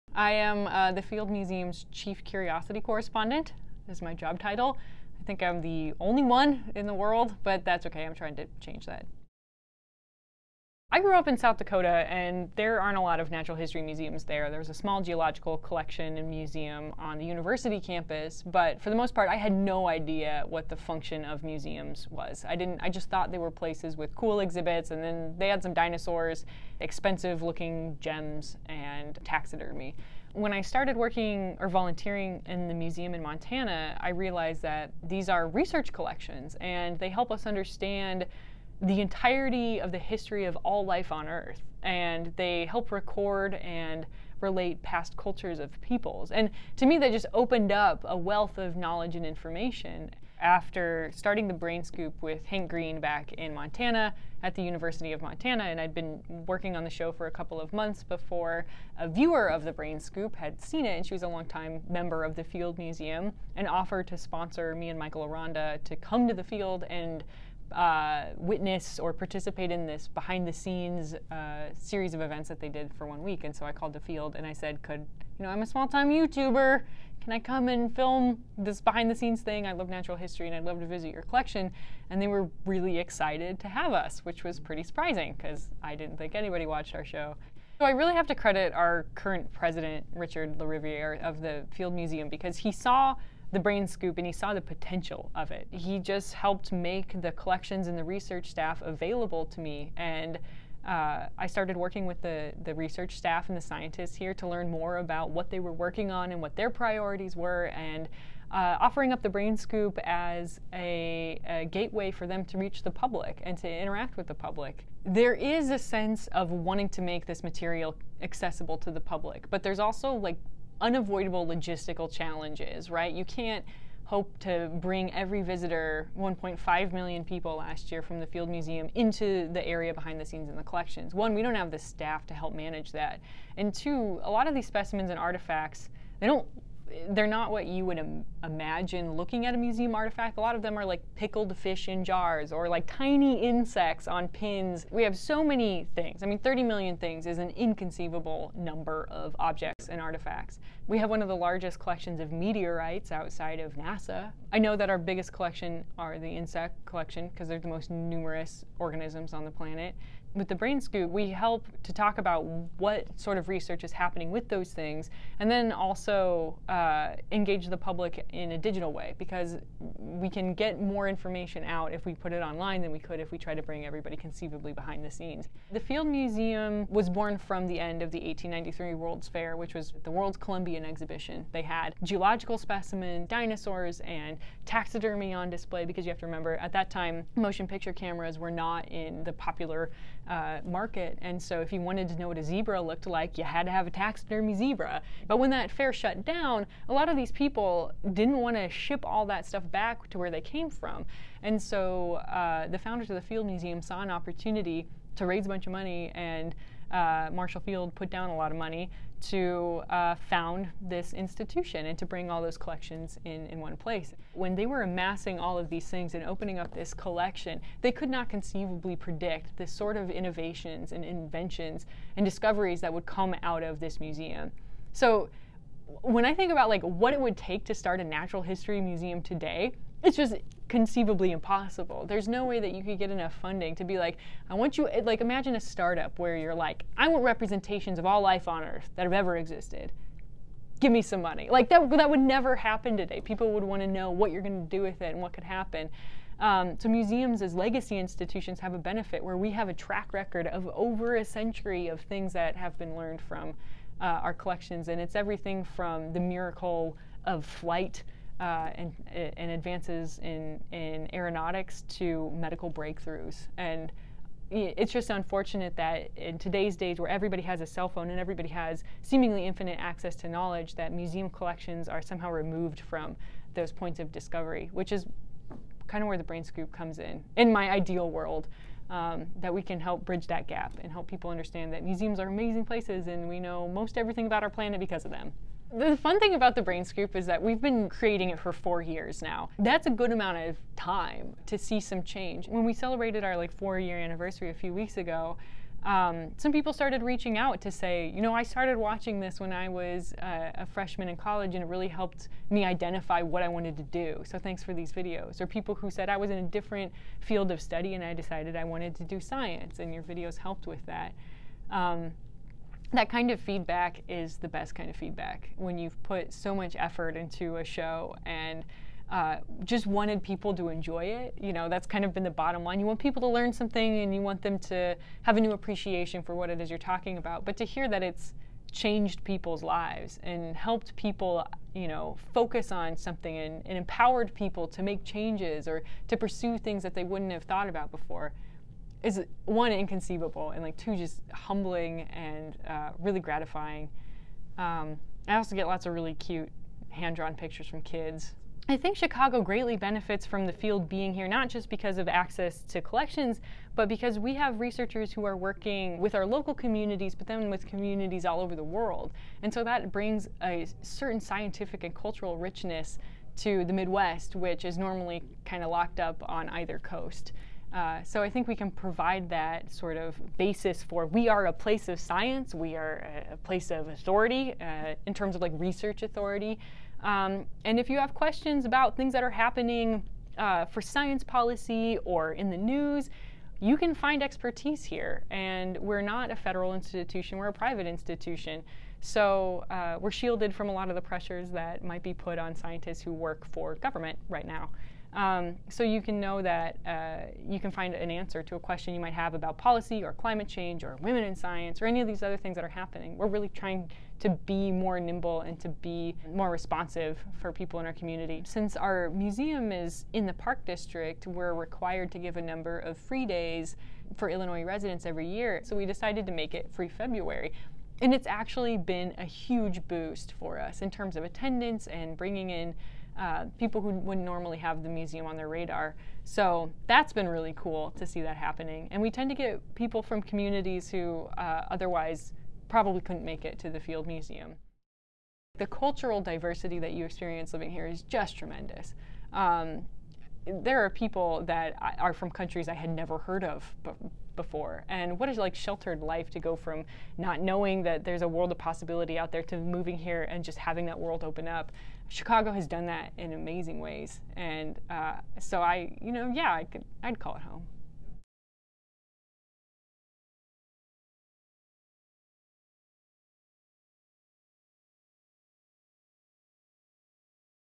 Chicagoans - Emily
Emily Graslie is the Chief Curiosity Correspondent at The Field Museum. She hosts the YouTube channel The Brain Scoop which explores the museums collection and brings it to life.

Gear
Main Camera

Film by the Chicago Aussie